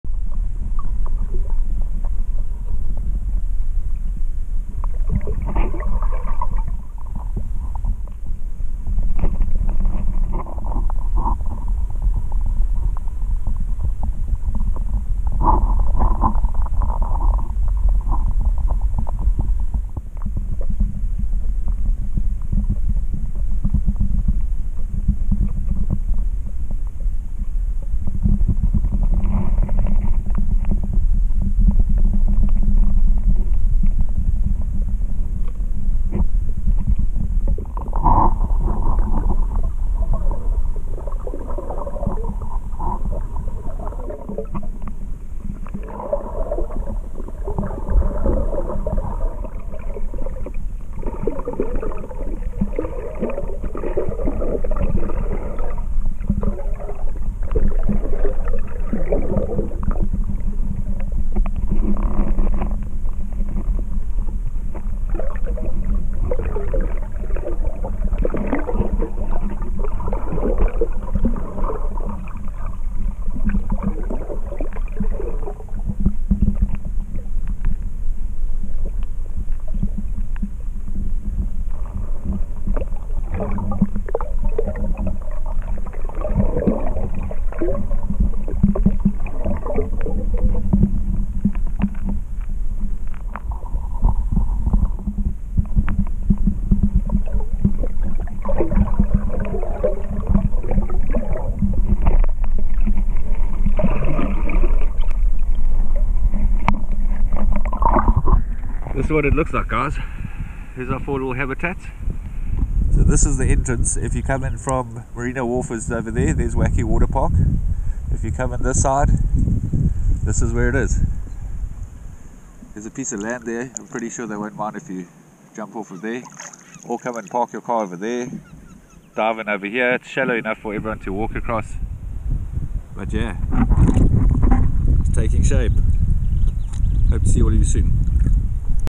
Marina Martinique Fish Habitat snorkel route
Small fish starting to get to know the new habitats